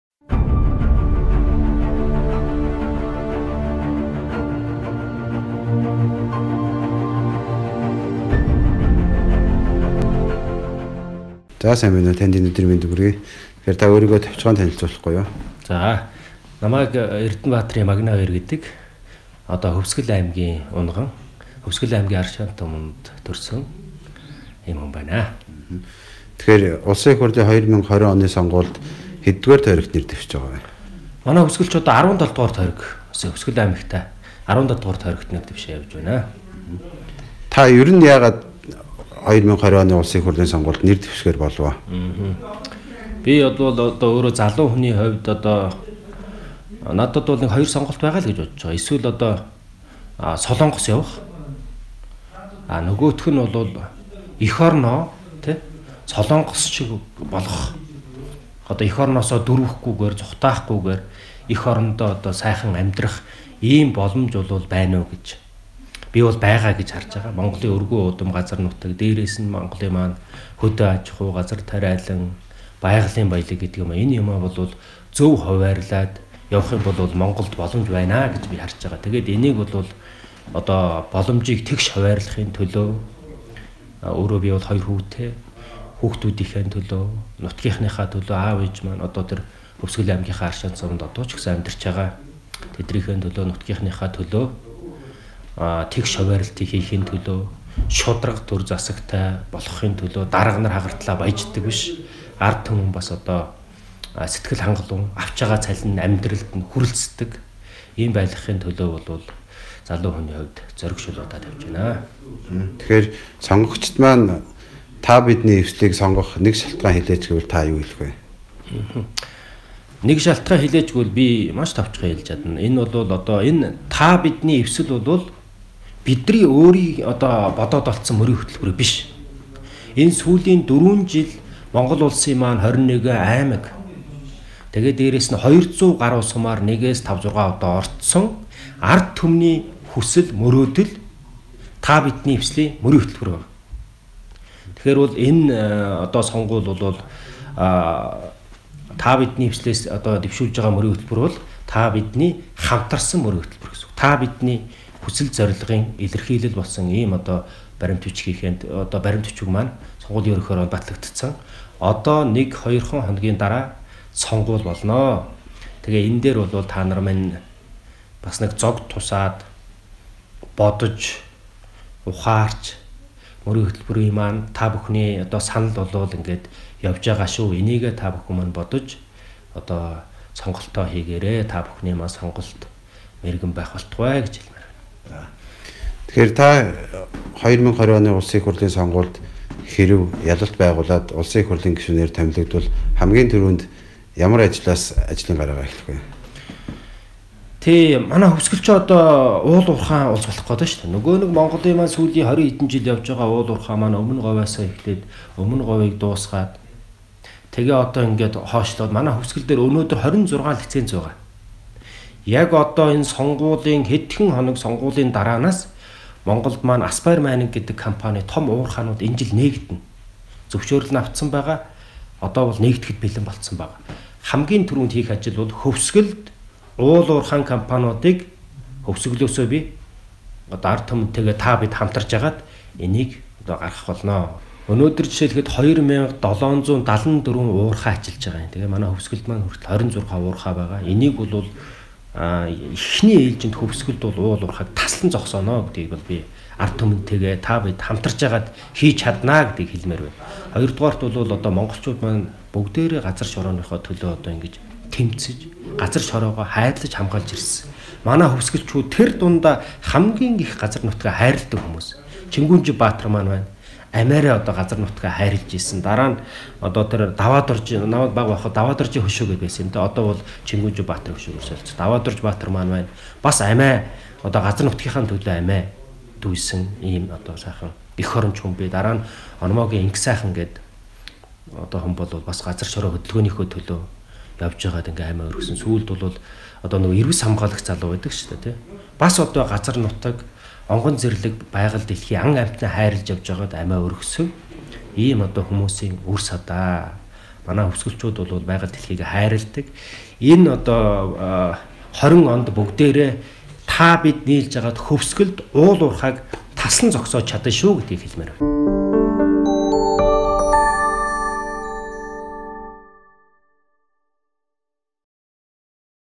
Э.Магнайбаяр: Хөвсгөл аймагт байгаа уурхайнуудыг "Та, бид" хамтран гаргана
УИХ-ын 2020 оны сонгуульд "Та, бидний эвсэл"-ээс нэр дэвшигч Э.Магнайбаяр